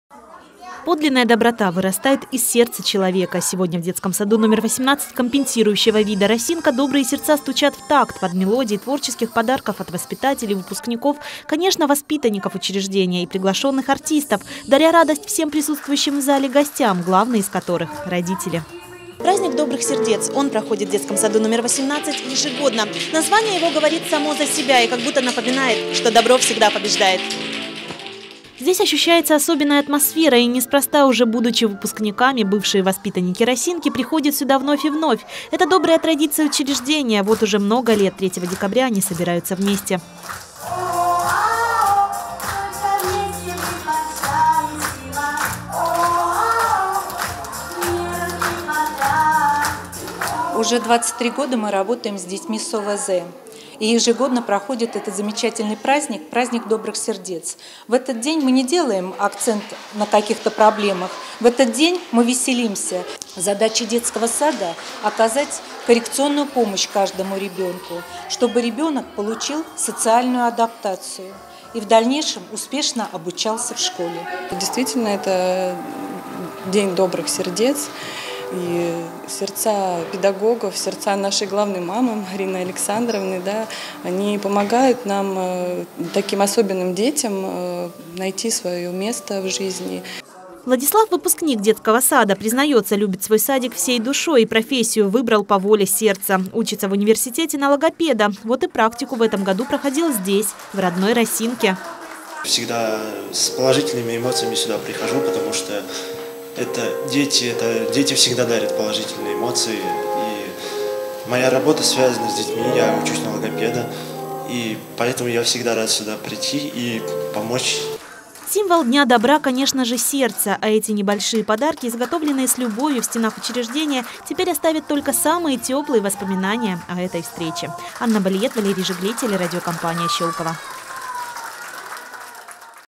Даря душевное тепло.
Праздник добрых сердец, приуроченный к международному дню инвалидов, прошел в детском саду №18 «Росинка» компенсирующего вида. Его по традиции учреждение отмечает вот уже много лет.